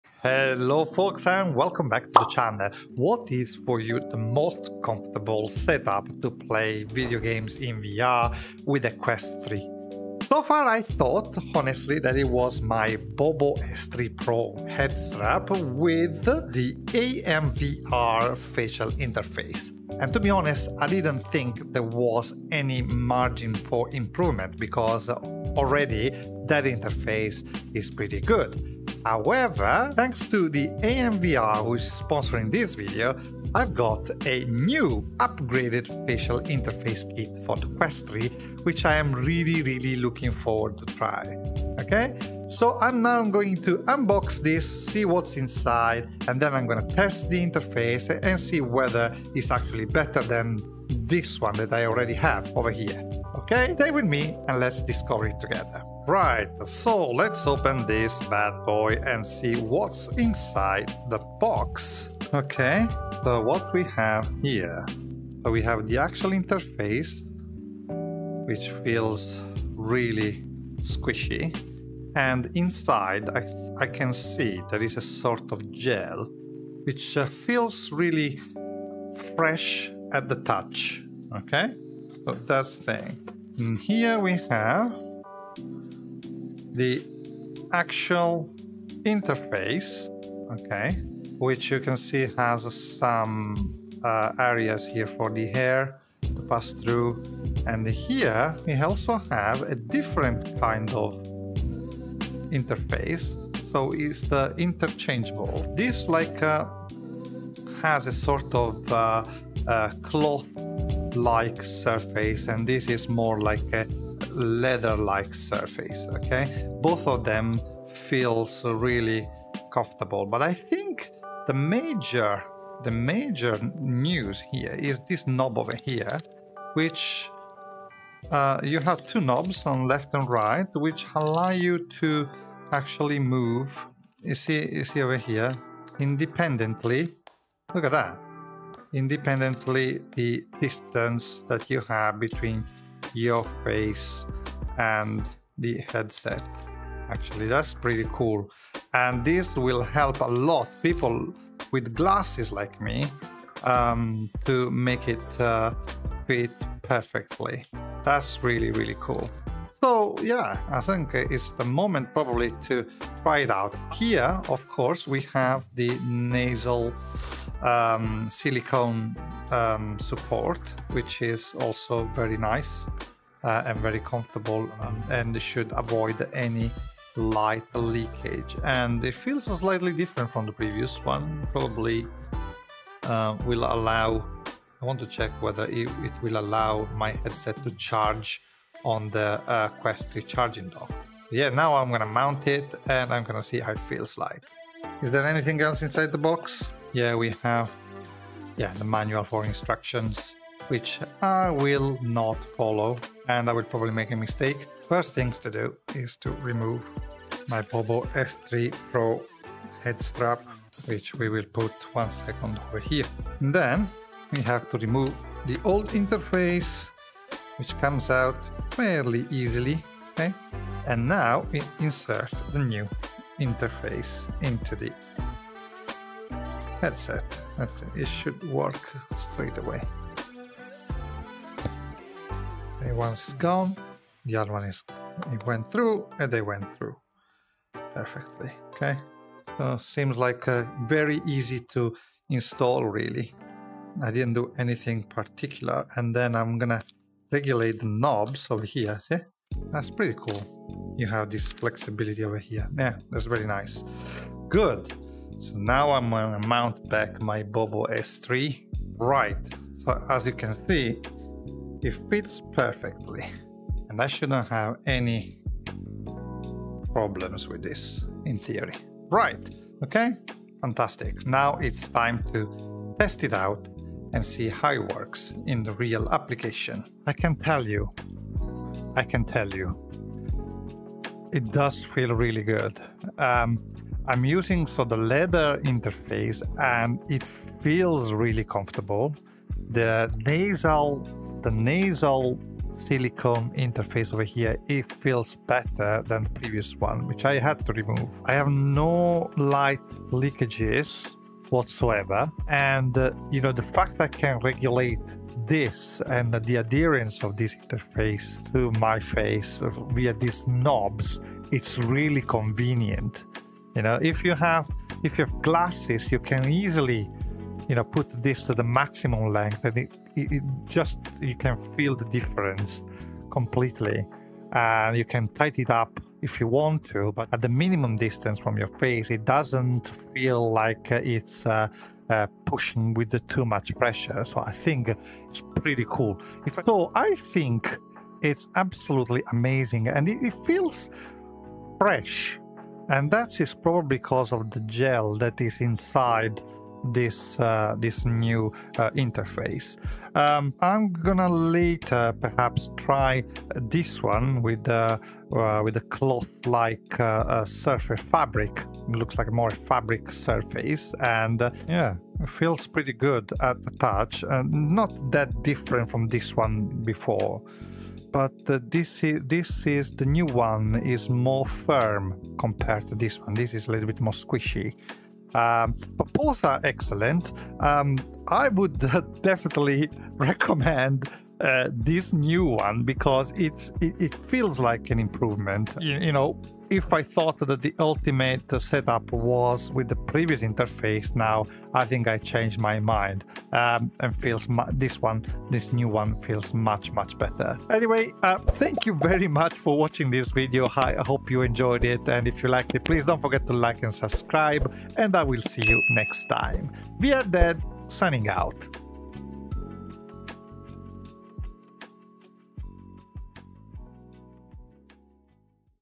The NEW Quest 3 Accessory That Will CHANGE Your VR Experience!
The BEST Facial Interface for Quest 3: Unboxing and Testing of the AMVR Upgraded Facial Interface Kit for Quest 3.